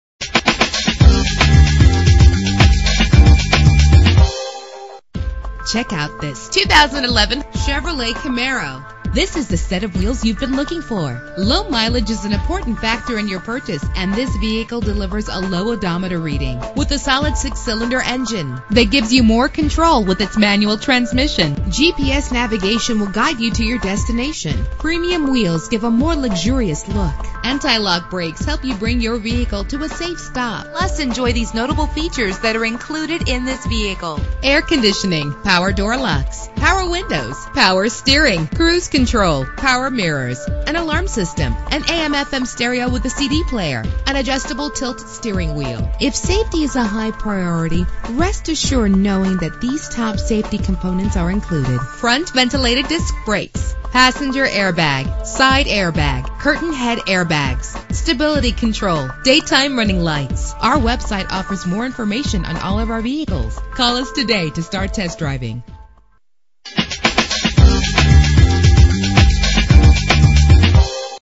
2011 Chevrolet Camaro - Wheat Ridge CO
Year: 2011
Make: Chevrolet
Model: Camaro
Trim: 1LS
Engine: 3.6 liter 6 cylinder 24 valve
Transmission: Manual
Color: Synergy Green Metallic
Mileage: 4000
Address:
11001 West I-70 Frontage Rd North